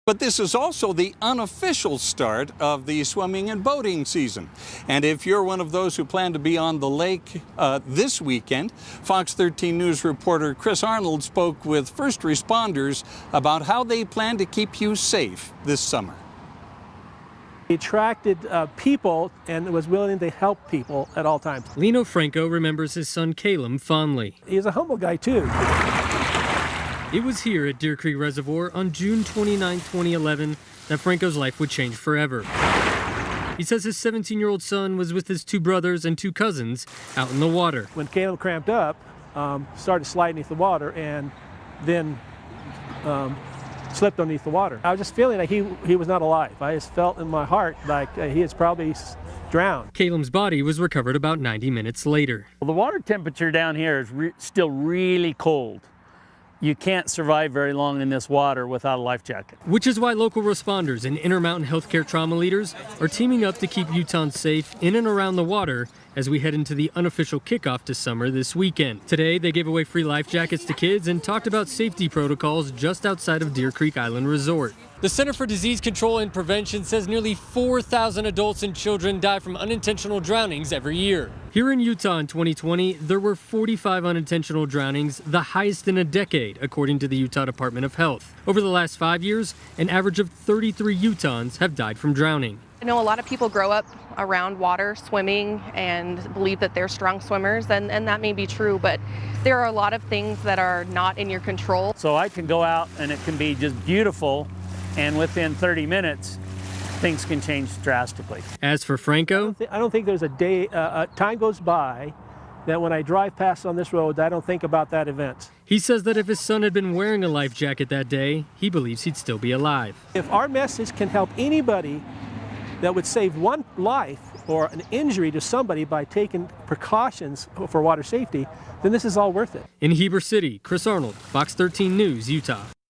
First responders, trauma leaders preach water safety heading into Memorial Day weekend
Memorial Day weekend signals the unofficial start to summer as well as boating and swimming season.

Thursday, local first responders and Intermountain Healthcare trauma leaders teamed up to try and raise awareness for water safety.

Out at Deer Creek Island Resort, free life jackets were handed out to kids and they talked about safety protocols at Deer Creek Reservoir, which tends to be a highly recreated during the summer months.